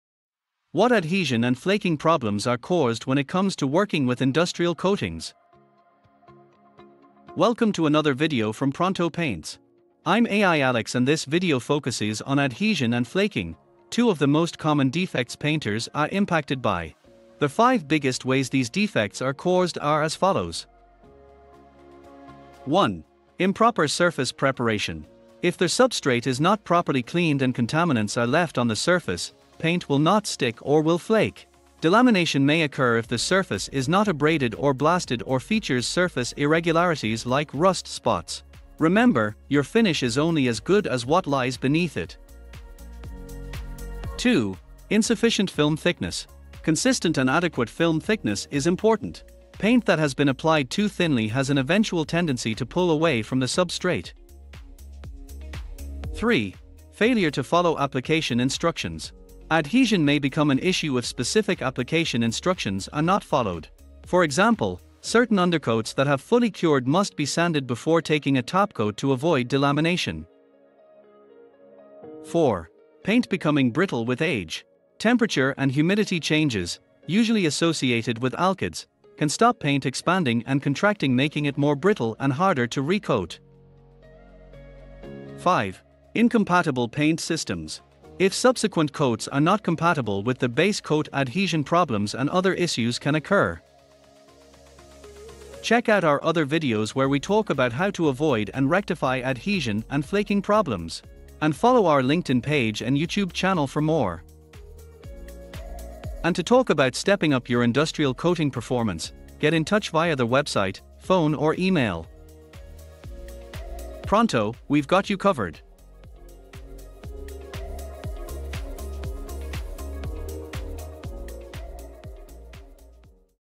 Paint Adhesion & flaking advice from Pronto Paints (1/3)
Pronto Paints video focusing on the problem of adhesion and flaking when using industrial coatings (part 1)